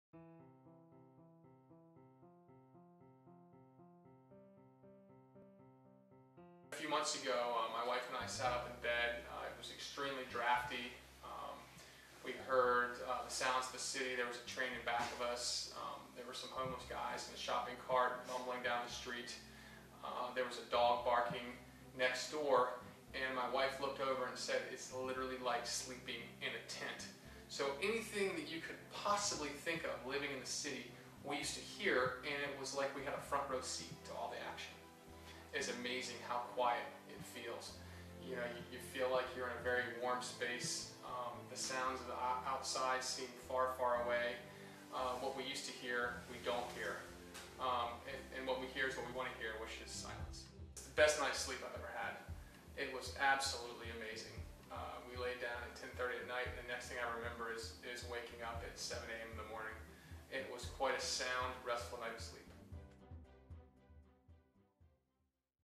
Blackout Windows in Your Bedroom with Indow Inserts
"Best night's sleep I've ever had," says this Indow customer after being kept awake by city noises for years. He installed blackout windows in his bedroom to block traffic noise, drafts, & all light to have sound, restful sleep.